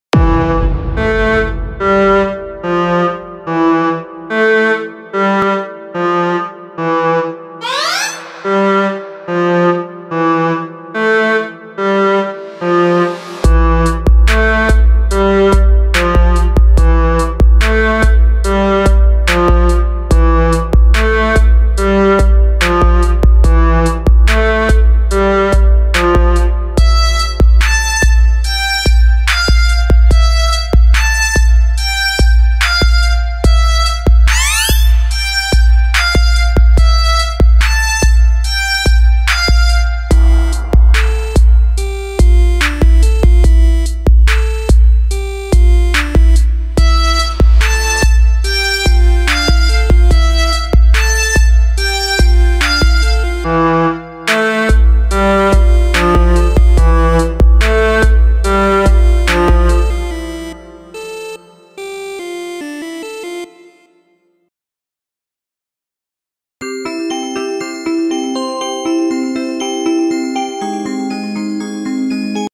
The Sad Beats➲Bollywood Style➲Mixing Abhi➲ really sad beats
THIS VIDEO IS ABOUT THE INSPIRATION FROM BOLLYWOOD NEW SONG VIDEO AND THERE SOME HOW I MAKE THIS TUNE PLEASE SUPPORT ME AND SUBSCRIBE TO THIS CHANNEL LIKE THIS VIDEO AND FOLLOW ME ON INSTAGRAM LINKS BELOW. THANKYOU